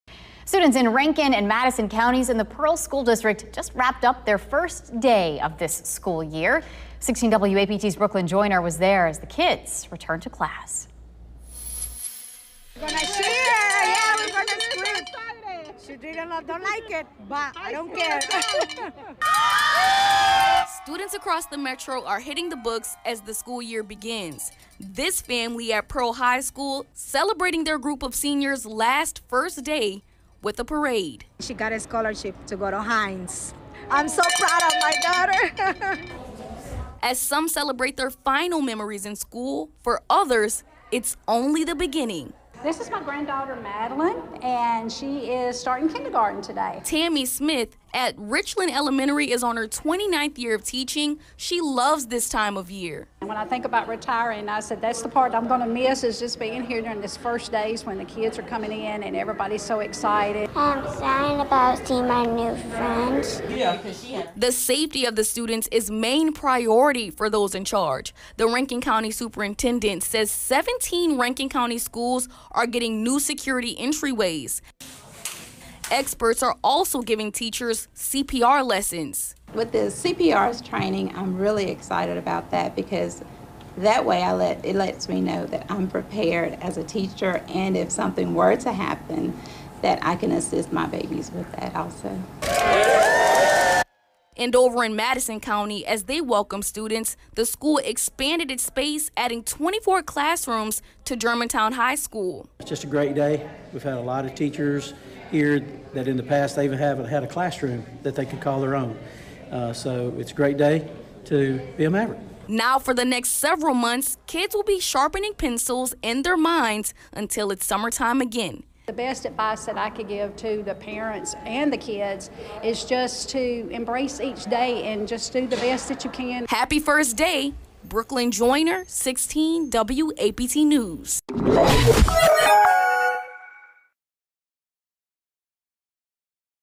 Rankin and Madison county students return to school
More metro students head back to class